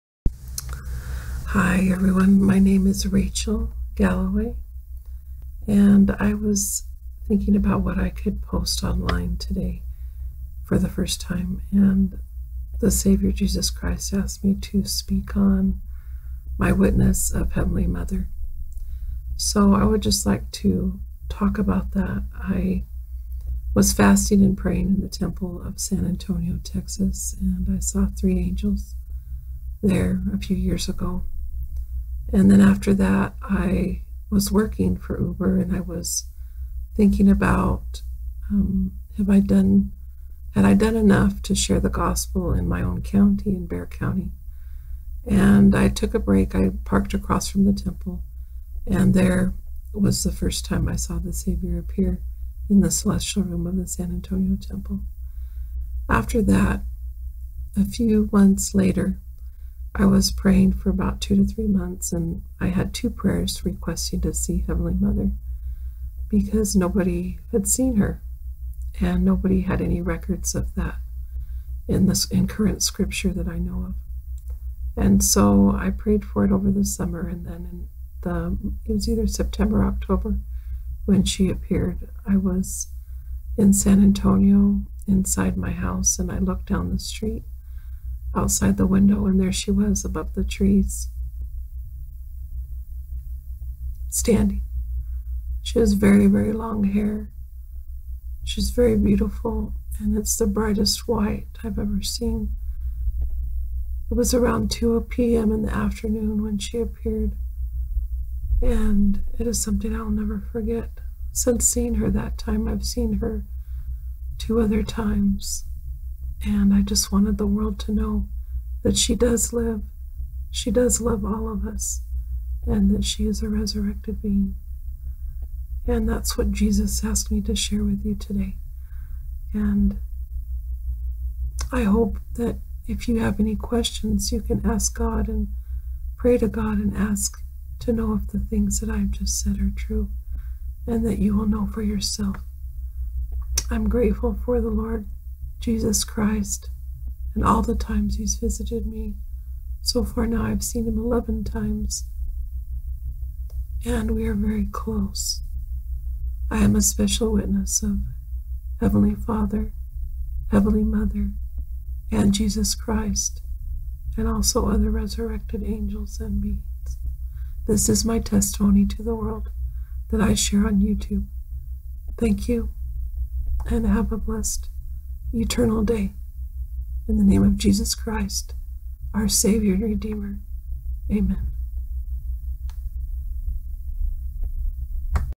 WIN 20220218 20 26 50 Pro personal witness of Goddess, Heavenly Mother
My personal witness of Goddess, or Heavenly Mother.